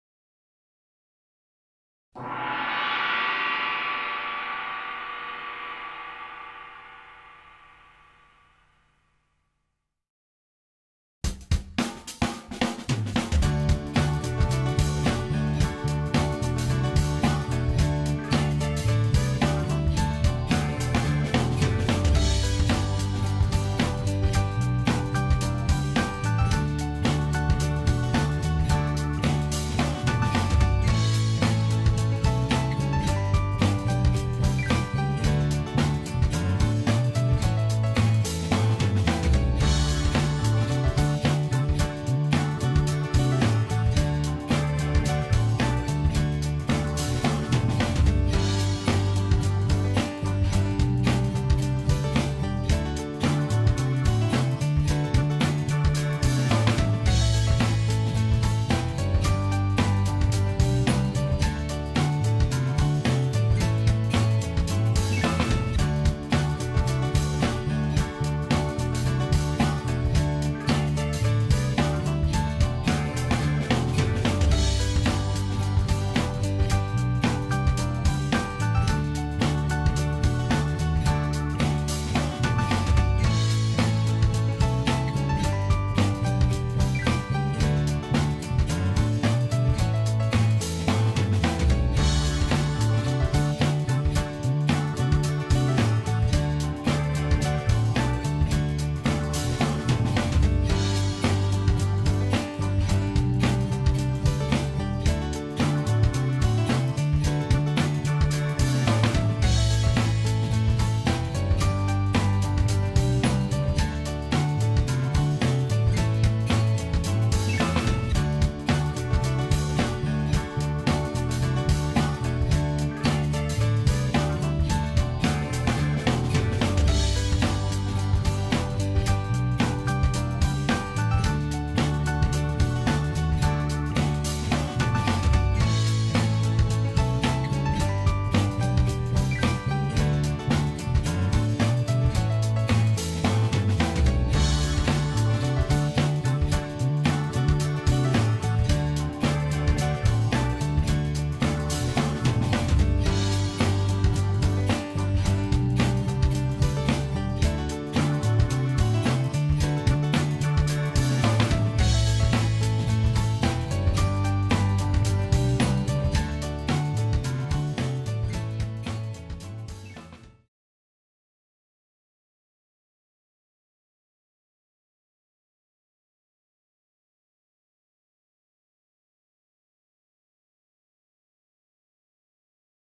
Piste Soutien (23) Country Blues. Moyen - vite. D majeur.
Un Blues animé avec l'orgue Hammond et le piano comme Bruce Hornsby. Pas trop difficile à jouet en D majeur. Trouvez tous les vidéos compris accords de guitare et guitare solo à